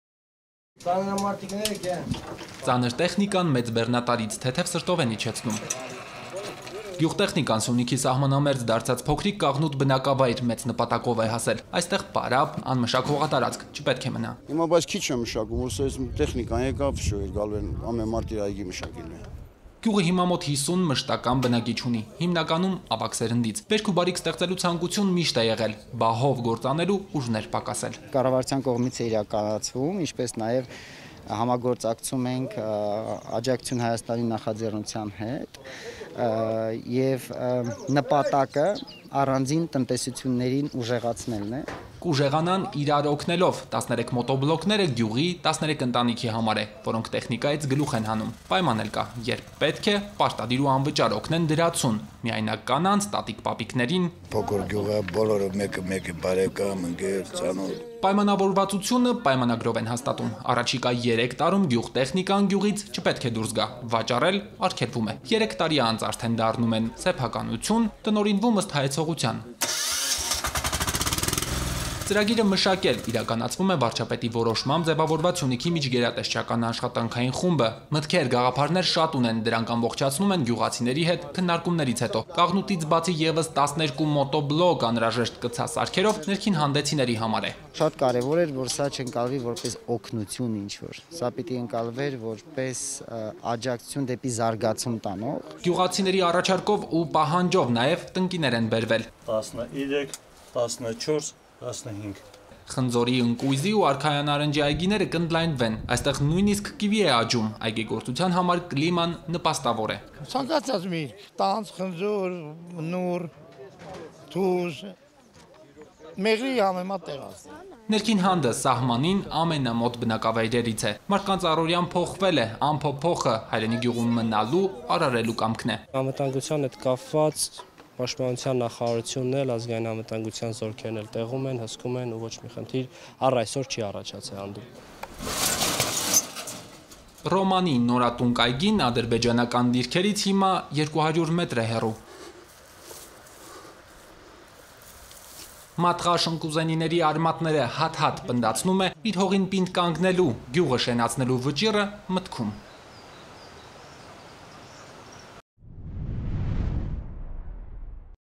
Կաղնուտի, Նոր Հանդի բնակիչները 25 գյուղտեխնիկա, 1000-ից ավելի տնկիներ են ստացել
Զարգանալ՝ միմյանց օգնելով: Այս ուղենիշով կառավարությունը սկսել է ծրագրեր իրականացնել Սյունիքի սահմանամերձ դարձած բնակավայրերում: Կաղնուտի և Նոր Հանդի բնակիչները 25 գյուղտեխնիկա, 1000-ից ավելի տնկիներ են ստացել՝ այգիներ հիմնելու ու անմշակ դարձած հողատարածքները նվազագույնի հասցնելու համար: Վարչապետի հանձնարարականով ձևավորված միջգերատեսչական խումբը ուղիղ կապի մեջ է բնակիչների հետ, այս ու սպասվելիք բոլոր ծրագրերը կյանքի են կոչում միայն համատեղ քննարկումներից հետո: